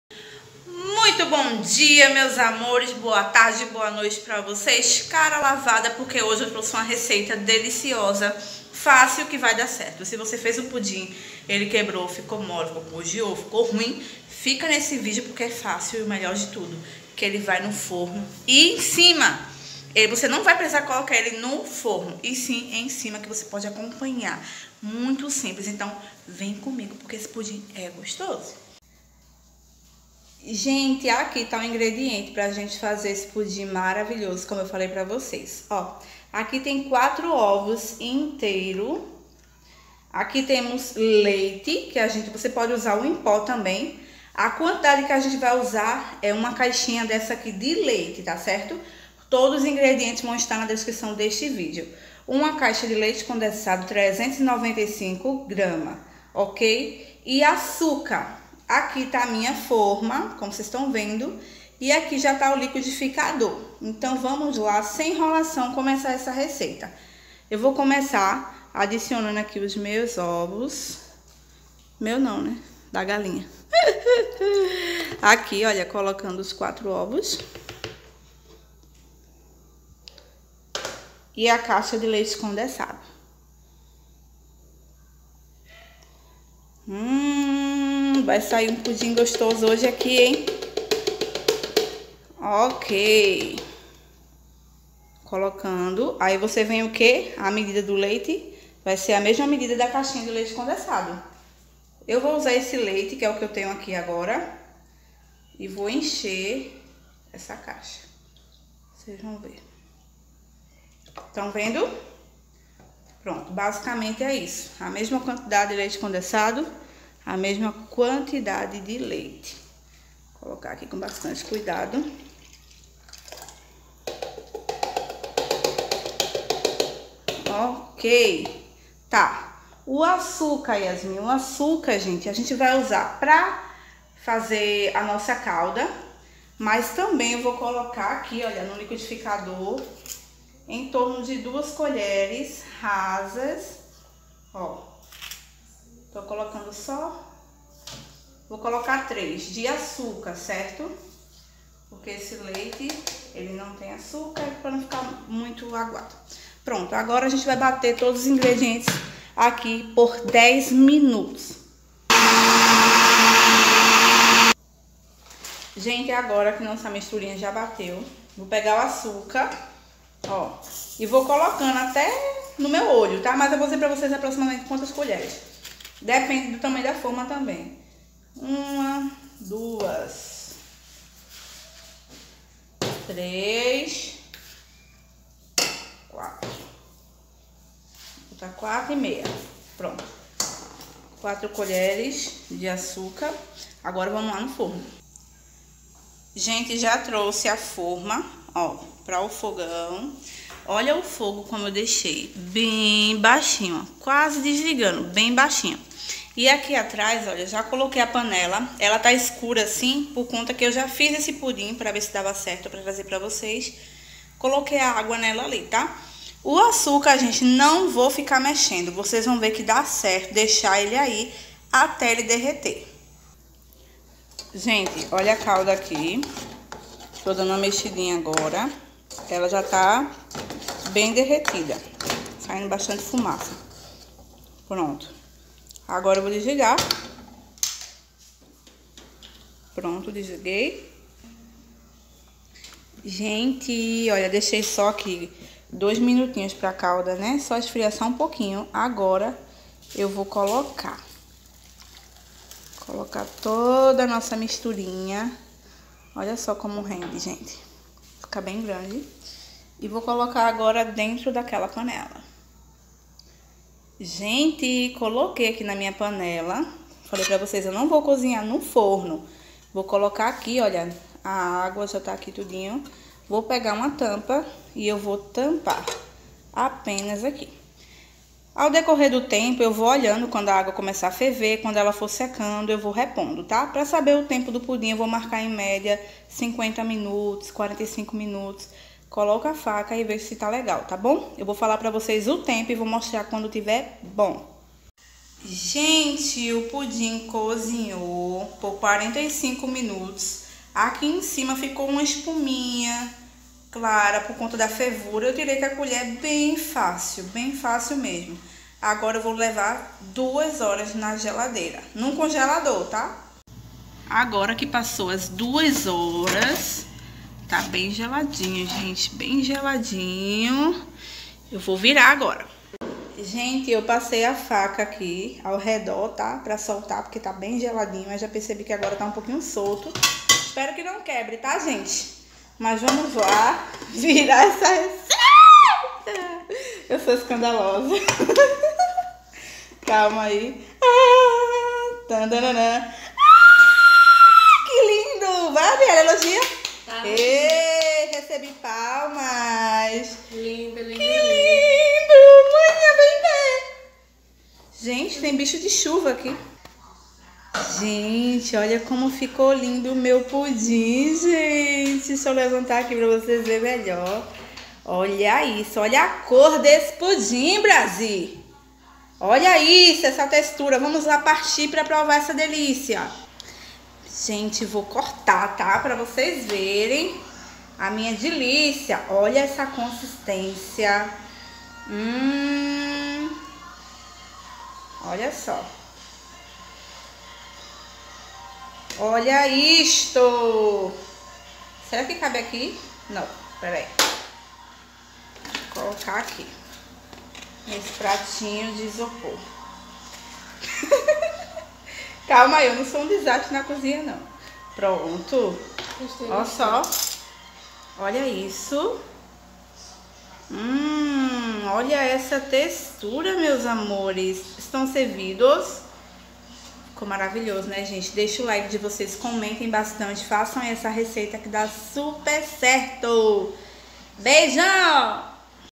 PUDIM SEM FORNO, SEM GELATINA O MAIS FÁCIL E GOSTOSO!
MEUS AMORES HOJE TRAGO PRA VOCES UMA RECEITA DELICIOSA DE PUDIM. E O MAIS INTERESSANTE, É QUE ELE NÃO VAI AO FORNO ELE NÃO LEVA GELATINA. ELE É FEITO EM UMA PANELA EMCIMA DO SEU FOGÃO.
VAMOS AOS INGREDIENTES DESSE PUDIM:
1 CAIXA DE LEITE CONDESSADO
1 CAIXA DE LEITE, A MESMA MEDIDA DA CAIXA DO LEITE CONDESSADO
4 OVOS INTEIRO
3 COLHERES DE AÇUCAR
PARA A CALDA: 4 COLHERES DE AÇUCAR.
VAMOS AO PASSO Á PASSO!
No liquidificador, adicione o leite condessado, e a caixa de leite. em seguida os ovos e as 3 colheres de açucar. bata tudo no liquidificador por 10 minutos.
É importante bater por 10 minutos, para não ficar nenhum cheiro de ovo.
enquanto bate, você adiciona as 4 colheres de açucar na forma, e leva para o fogo bem baixinho, para derreter todo açuca. quando atingir o tempo de batimento no liquidificador, pode adicionar na forma. Pegue uma panela que seja maior que a forma e adicione 2 copos de água, em seguida coloque sua forma dentro, é opicional tampar apenas a forma. leve ao fogo por 45 minutos á 1 hora isso depende de cada fogão.
você pode verificar se estar cozinhado, colocando uma faca, se sair limpa é porque ja estar bom.
espere esfriar por 40 minutos e leve para o congelador, por 2 horas. agora sim estar pronto para servir, mas uma dica, passe a faca nas laterais para facilitar e não quebrar.
espero que tenha gostado da receita.